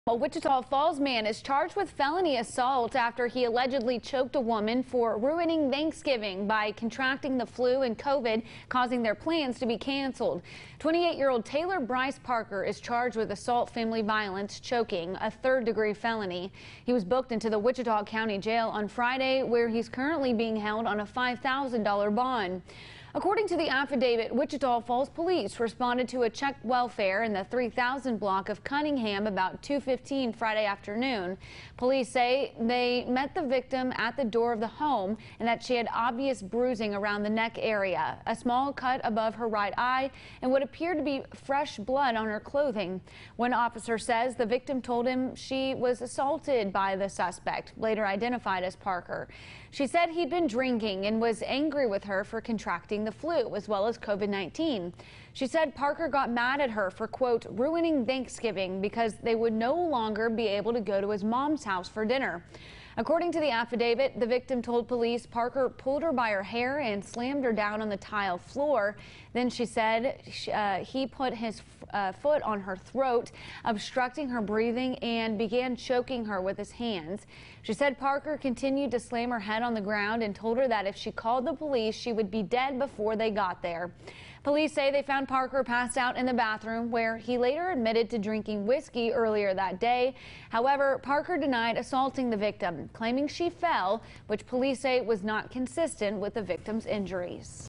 Man allegedly assaults woman for 'ruining Thanksgiving' by contracting COVID
A Wichita Falls man is charged with felony assault after he allegedly choked a woman and slammed her head on the ground for "ruining Thanksgiving" by contracting the flu and COVID-19, causing their plans to be canceled.